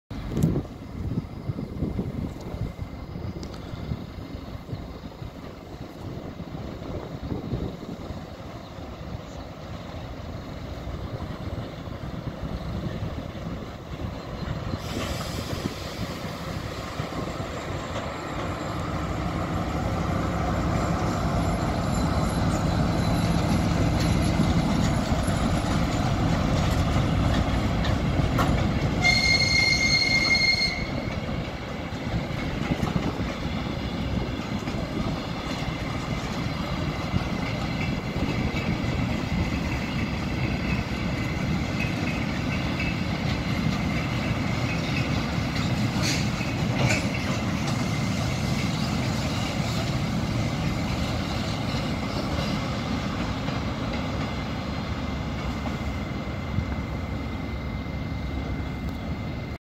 Locomotive M62-1701 with maintenance train going through main station Rīga-Pasažieru.
Railway station Riga-Passenger.
Riga, Latvia.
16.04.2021
13:15